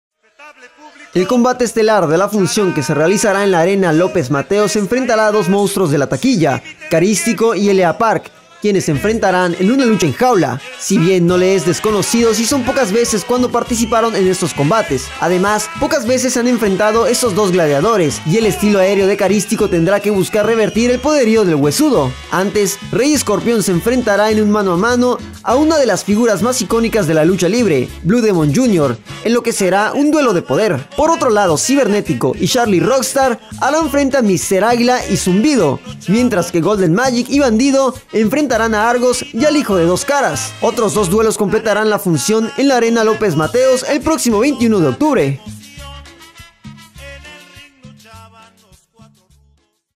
A la espera de un choque de ídolos de la lucha
El combate estelar de la función que se realizará en la Arena López Mateos donde se enfrentarán dos monstruos de taquilla, Carístico y L. A. Park, quienes se enfrentarán en una lucha en jaula.